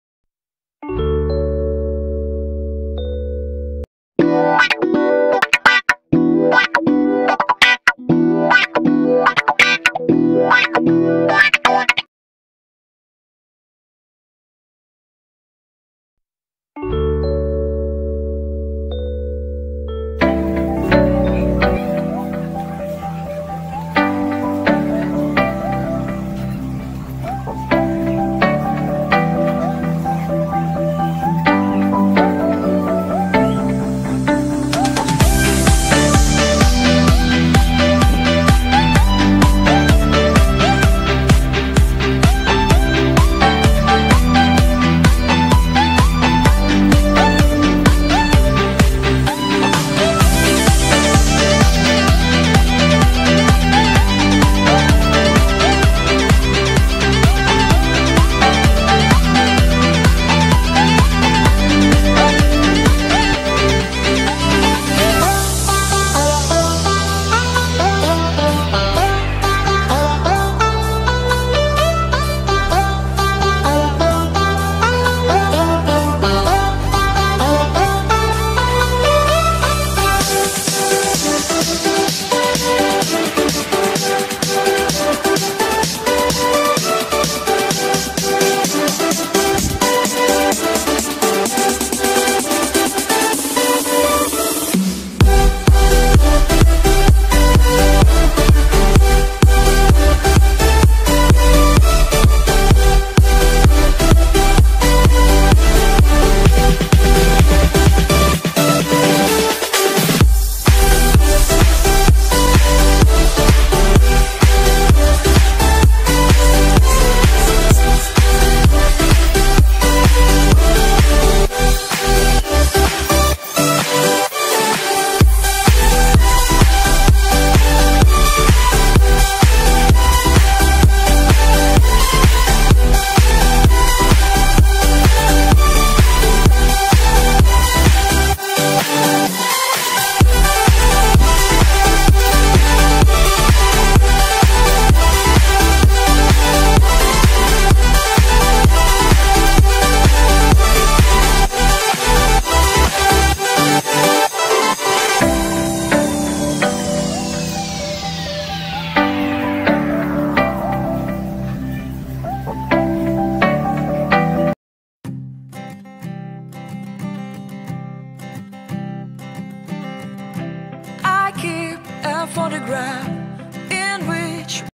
Drawing time, fashion dress, stress reliever
Materials: oil pastel, colored pencil, fine marker stabilo, art clay eraser.